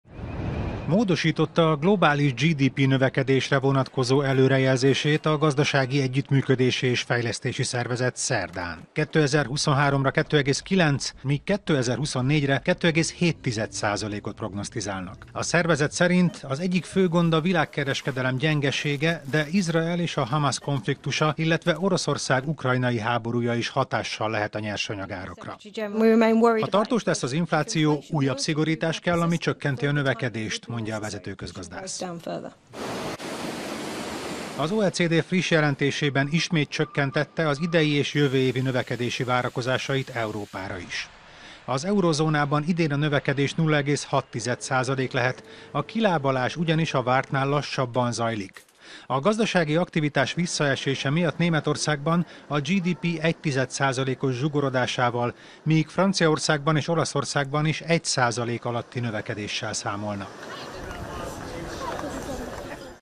2.9 százalékkal nőhet idén a világ GDP-je
Módosította a globális GDP-növekedésre vonatkozó előrejelzését Gazdasági Együttműködési és Fejlesztési Szervezet, amely 2024-re 2,7 százalékot prognosztizál.